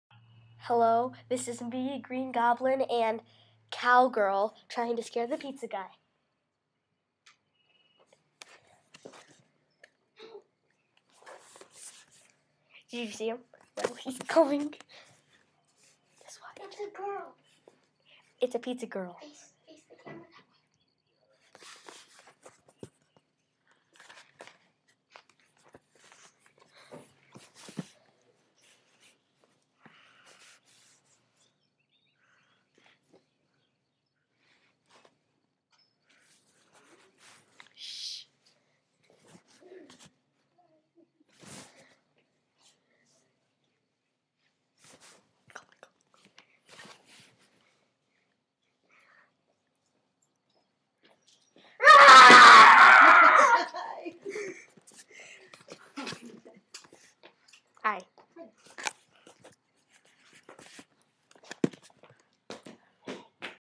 Pizza Delivery Scare
green goblin, cowgirl, and beanie dude scare the pizza girl